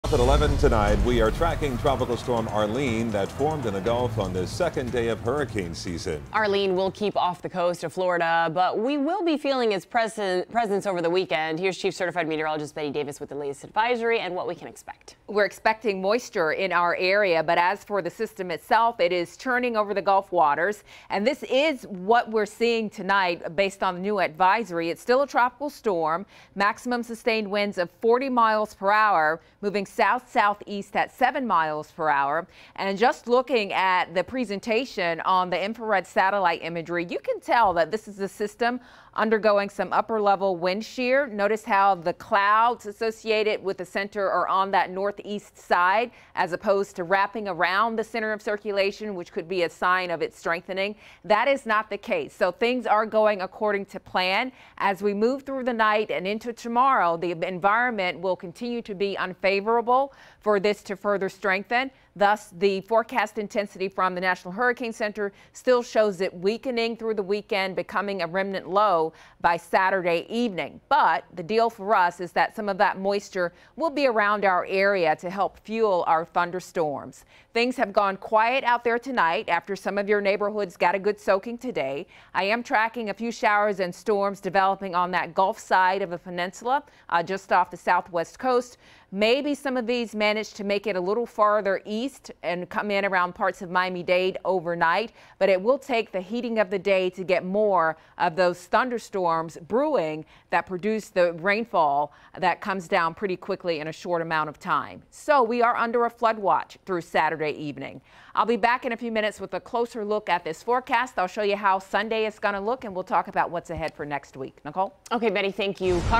Tropical Storm Arlene's moisture causes rain in South Florida
Tropical Storm Arlene's moisture was set to cause rain again on Saturday in South Florida.